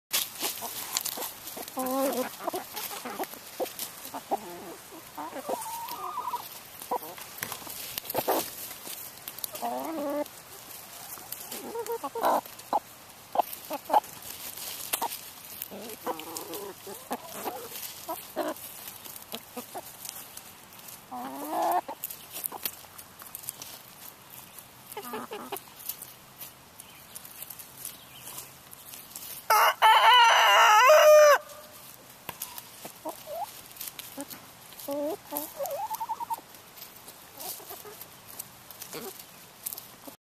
chickens eating greens with abandon
chickens at GB's place in Gill, MA.